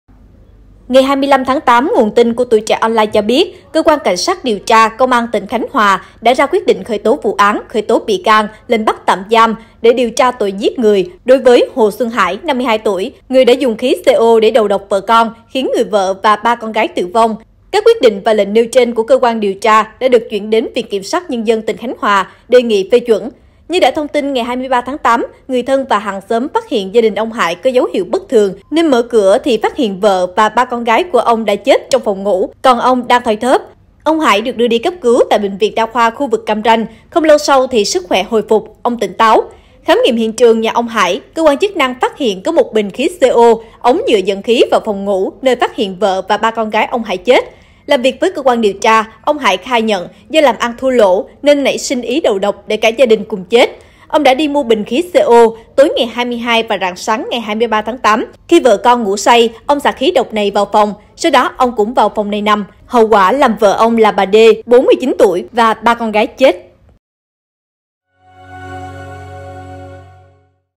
Khởi tố người chồng đầu độc khiến vợ và 3 con gái tử vong ở Khánh Hòa
Cơ quan cảnh sát điều tra Công an tỉnh Khánh Hòa đã ra quyết định khởi tố bị can, lệnh bắt tạm giam để điều tra tội 'giết người' đối với người đàn ông dùng khí CO đầu độc vợ con làm 4 người chết.